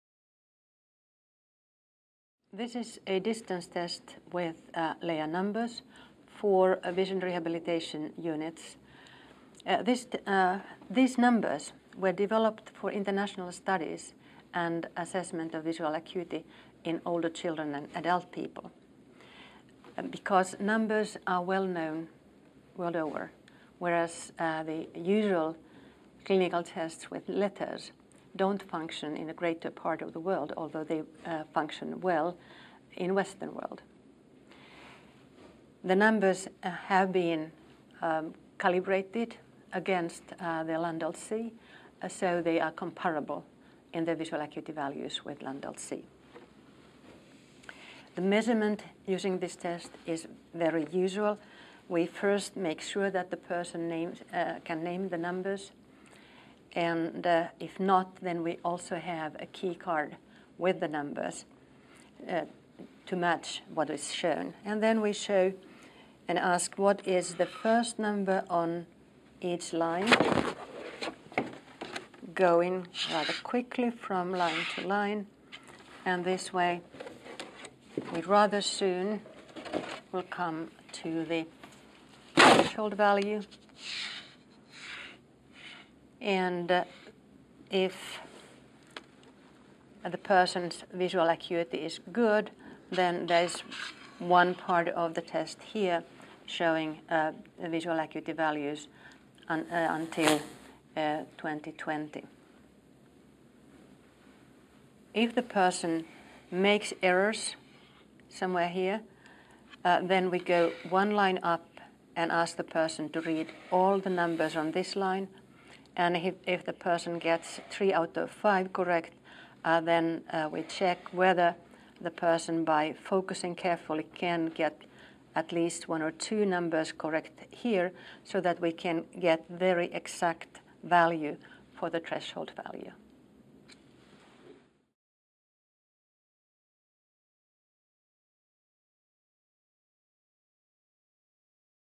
LEA NUMBERS® Chart for Vision Rehabilitation
Dr Lea Hyvarinen show you how to use this chart that is designed for testing children and adults with visual impairment or low vision.